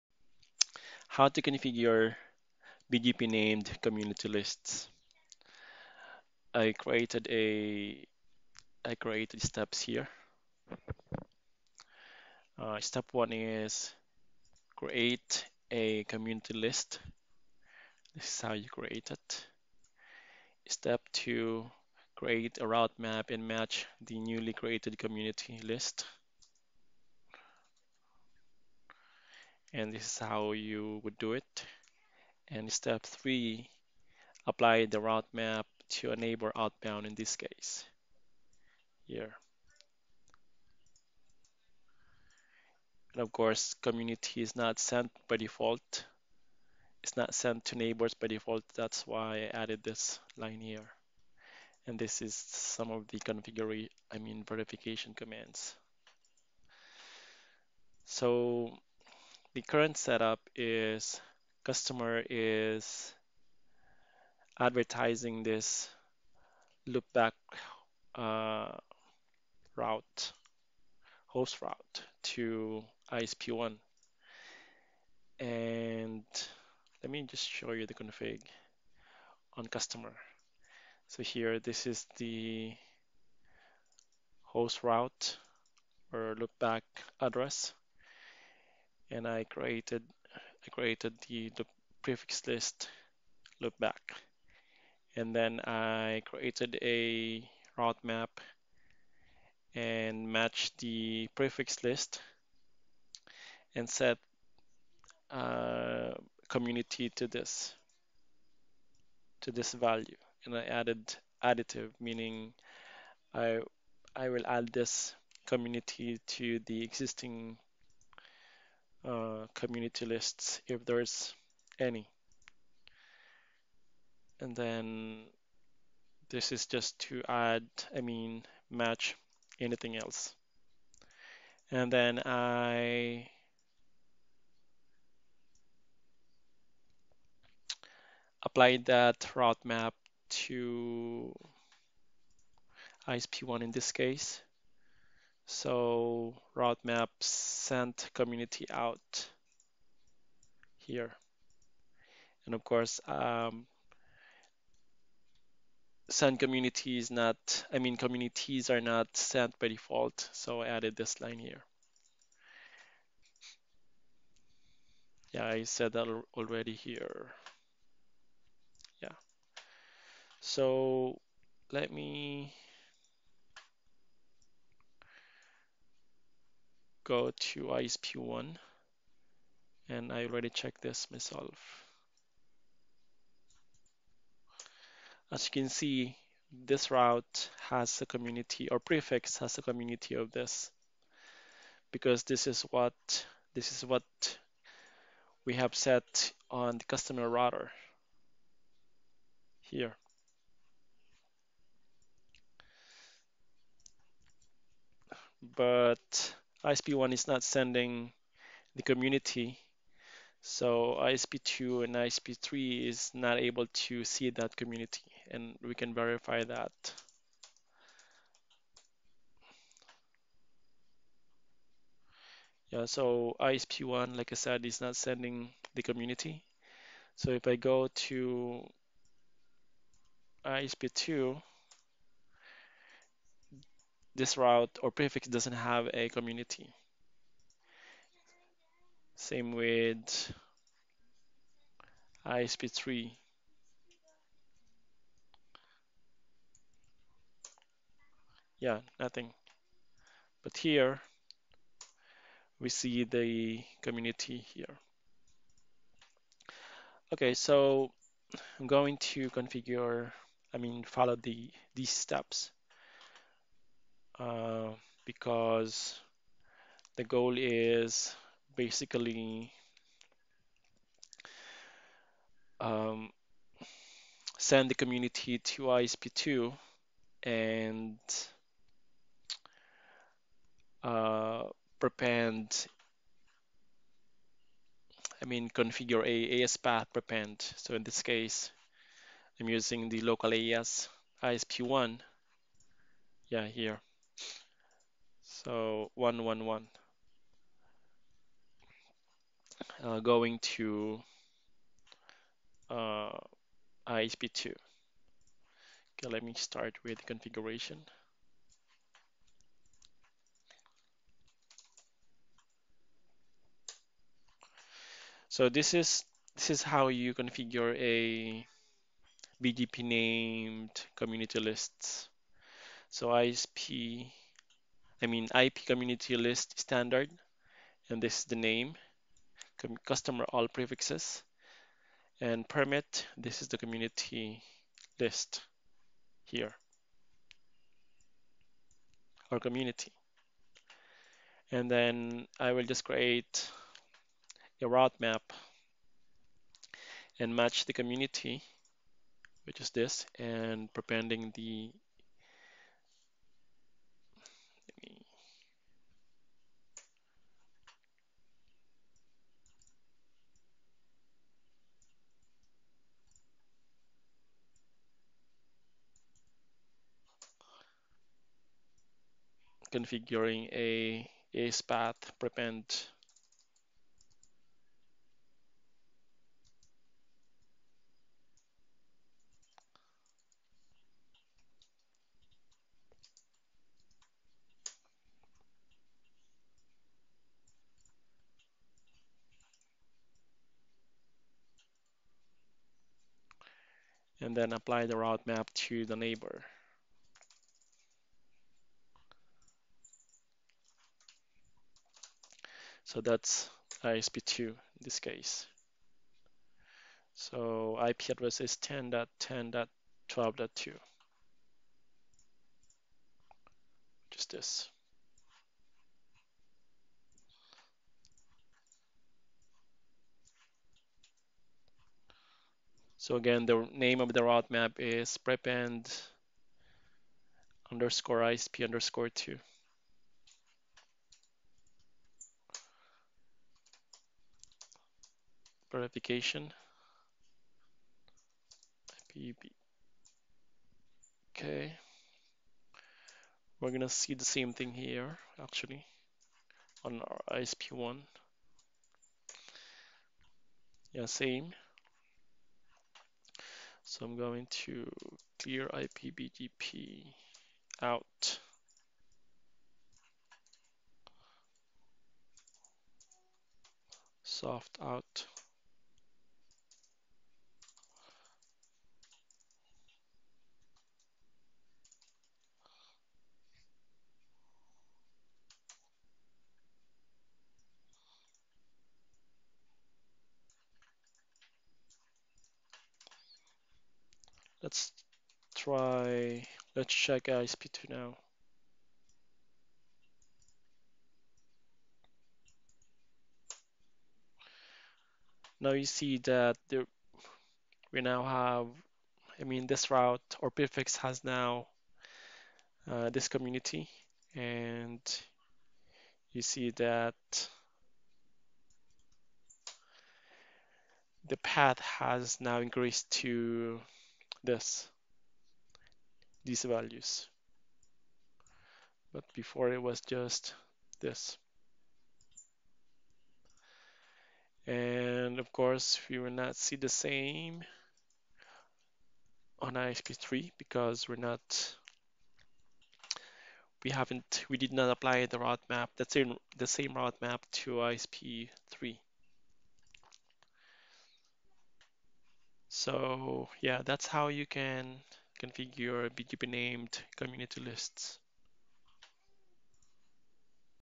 Configure BGP Named Community Lists | Step-by-step Tutorial
⌚ TIMESTAMPS
0:00 Introduction
4:16 Configuration
7:13 Verification
9:01 Conclusion

Learn how to configure BGP Named Community Lists in this step-by-step tutorial

Step 1: Create a community list.
conf t
ip community-list standard Customer_All_Prefixes permit 64512:0
Step 2: Create a route-map and match newly created community list.
route-map PREPEND_ISP_2 permit 10
 match community Customer_All_Prefixes
 set as-path prepend 1 1 1
route-map PREPEND_ISP_2 permit 20
Step 3: Apply route-map to a neighbor outbound in this case.
router bgp 1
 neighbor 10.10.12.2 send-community
 neighbor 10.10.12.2 route-map PREPEND_ISP_2 out

Verify:
show ip community-list
show ip bgp
show ip bgp 1.1.1.1
clear ip bgp * soft out

References:
BGP Named Community Lists: